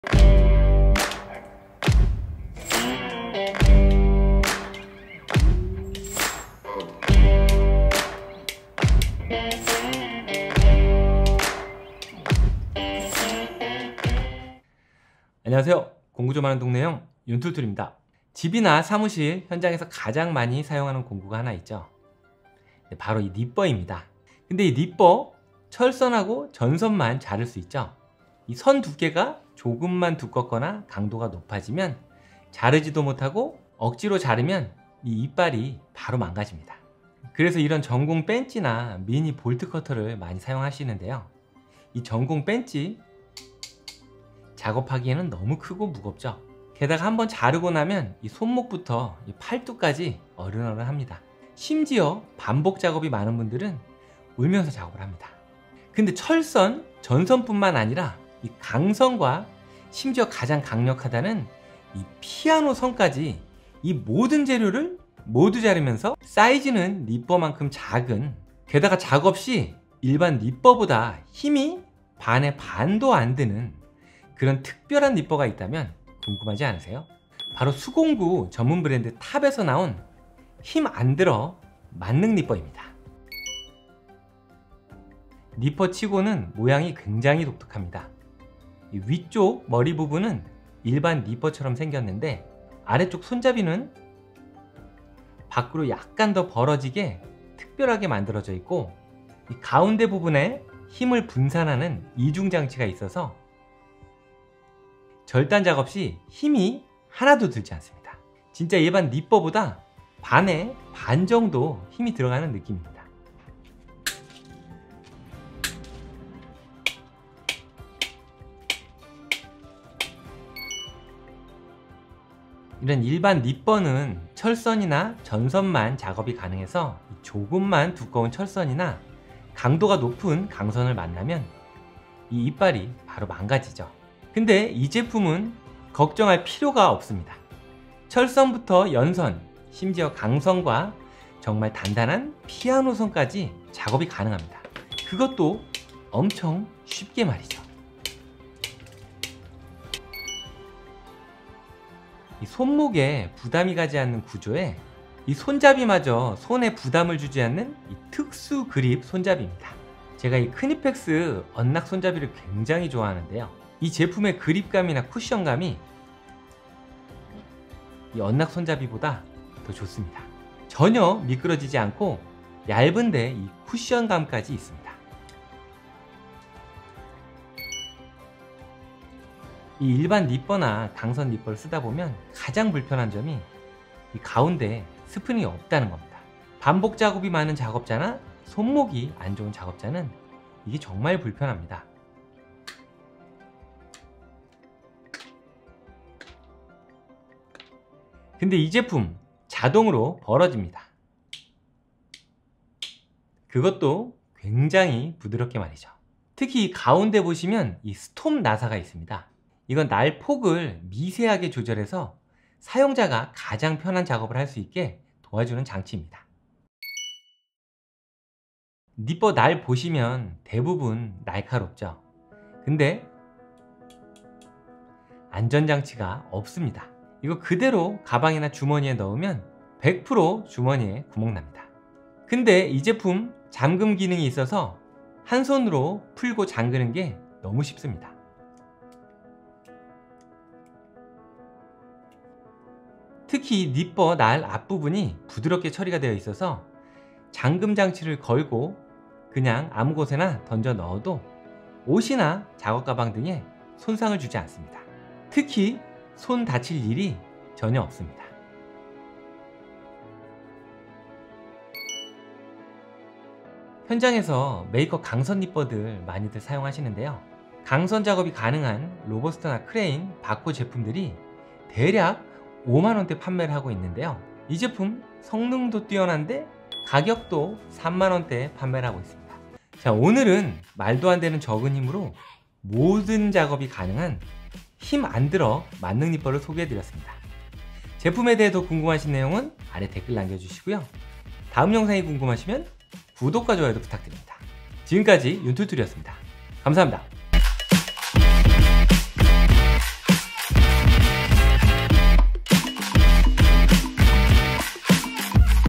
요즘에 이런 니퍼가 대세야? 자를 때 힘 안 드는 니퍼의 끝판왕! 세상에 없던 신개념!
구매처는 아래 링크 누르시면 됩니다

설명

세상에서 가장 힘 안드는 니퍼를 소개합니다!

영상으로 확인해 주세요 👈

📌 Time Stamp
0:00 - 인트로
0:15 - 세상에서 가장 힘이 안 드는 니퍼를 소개합니다
1:17 - 탑 ‘힘 안 들어’ 파워 니퍼가 특별한 이유👈
4:40 - 더 좋은 아이템으로 찾아오겠습니다